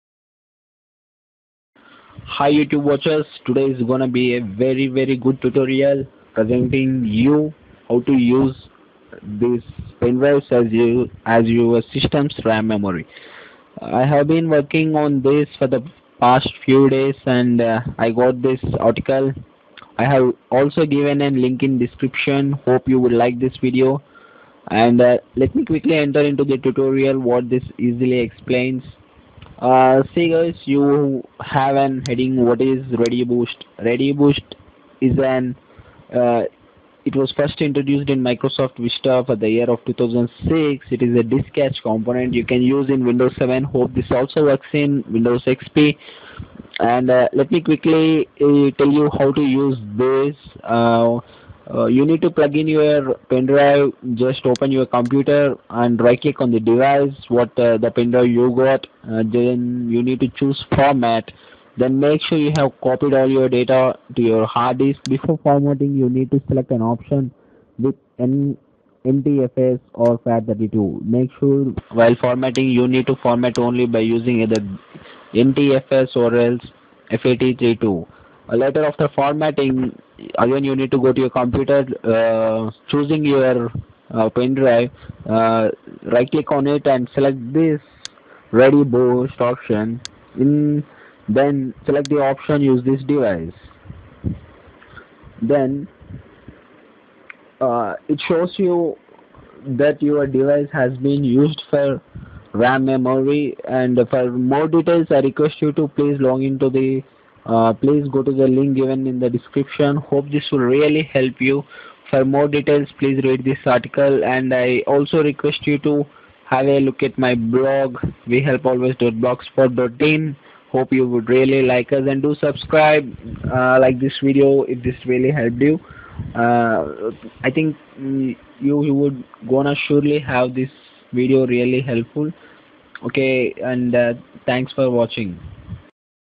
Using Pendrives As Ram Memory In Window7 HD VOICE NARRATED
This Videos Helps you to use your pendrives as your ram memory. This is really going to help you. Make sure you have windows7. Hope it also works in windowsxp not tested it winodws xp.

the above link really helps you.